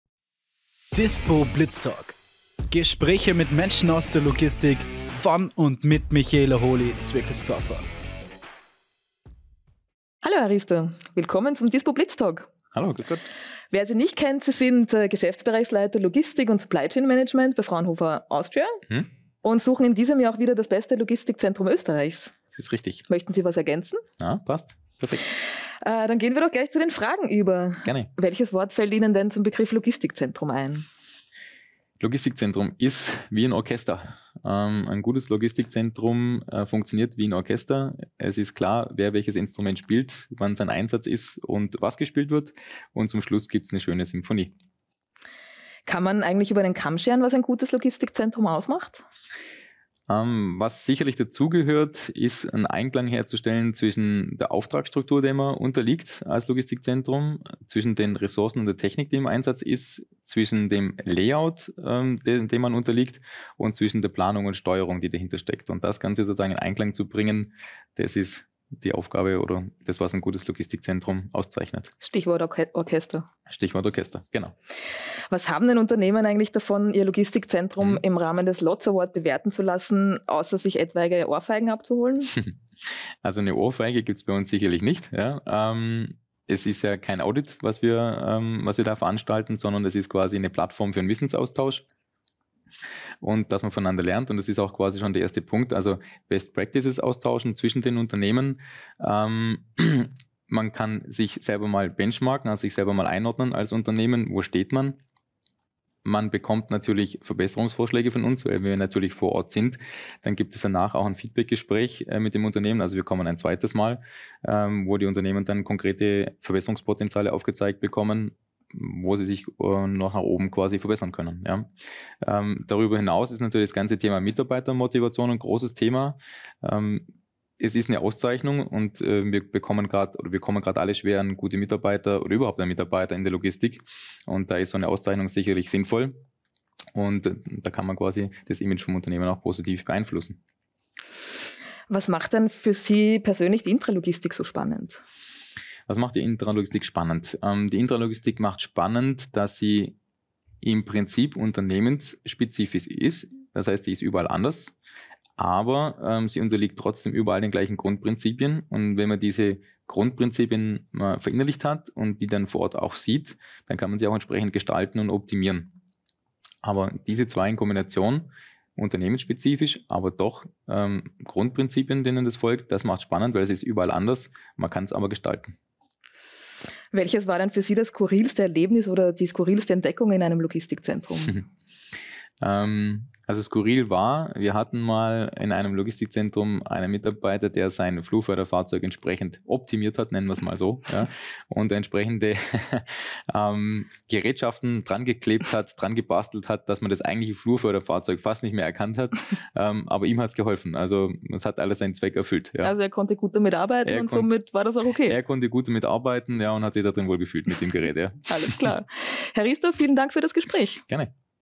Was macht ein gutes Logistikzentrum aus? Markus Riester von Fraunhofer Austria | dispo Blitztalk
Im heutigen Dispo Blitz Talk spricht Michaela Holy-Zwickelstorfer mit Markus Riester, Geschäftsbereichsleiter Logistik und Management bei Fraunhofer Austria, über die Bewertung von Logistikzentren durch den LOTZ Award. Riester vergleicht Logistikzentren mit einem Orchester, bei dem alles reibungslos zusammenarbeiten muss. Außerdem gibt er Einblicke in den Wissensaustausch und Benchmarking zwischen Unternehmen sowie eine spannende Anekdote aus der Intralogistik. Erfahrt mehr über die Bedeutung von Mitarbeitermotivation und Logistikoptimierung.

Schaut rein und erfahrt mehr über:

Den LOTZ Award
Erfolgsfaktoren für Logistikzentren
Best Practices und Mitarbeitermotivation in der Logistik